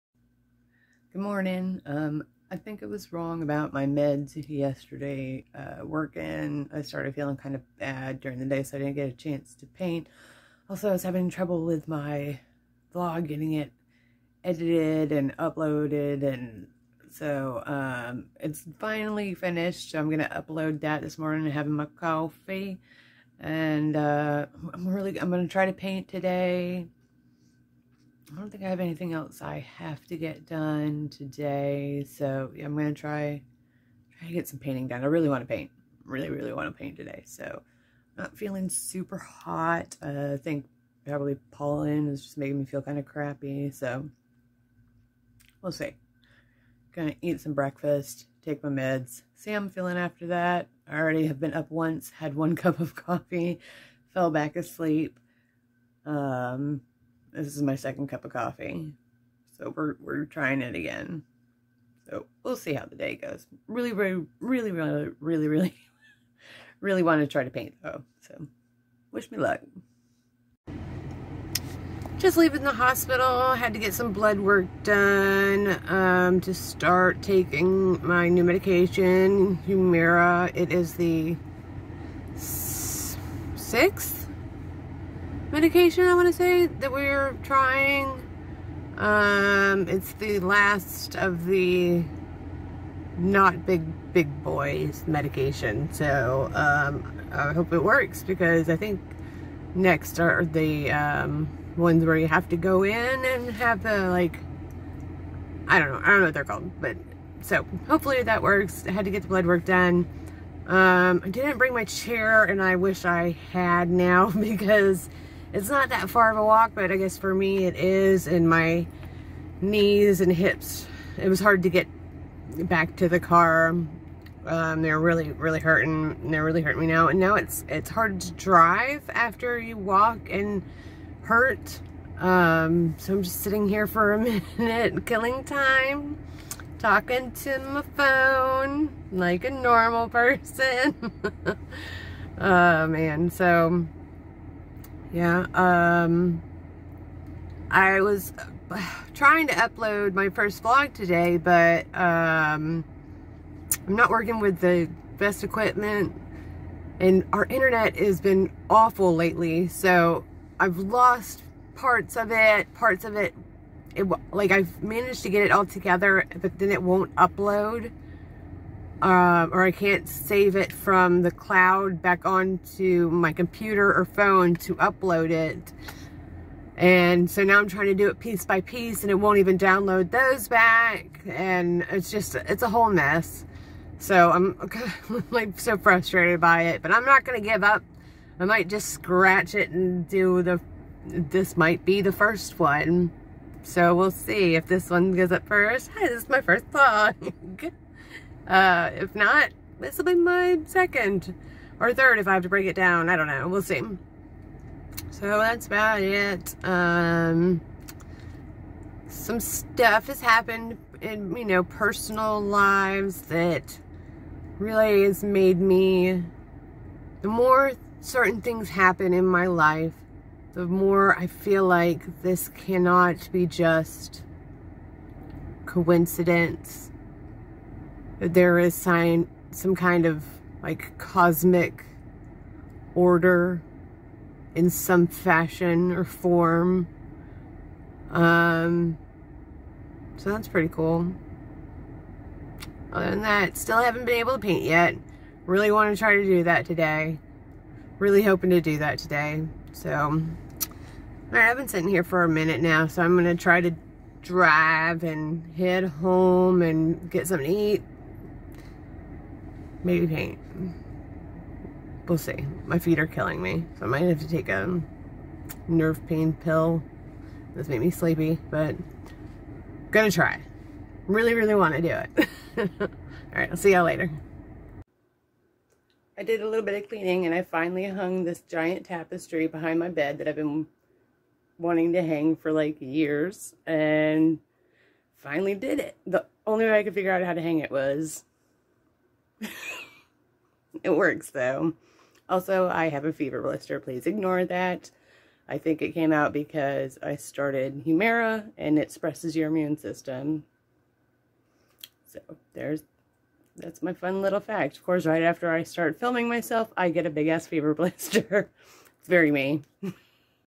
My life is often boring
Most days are uneventful and revolve around my chronic illness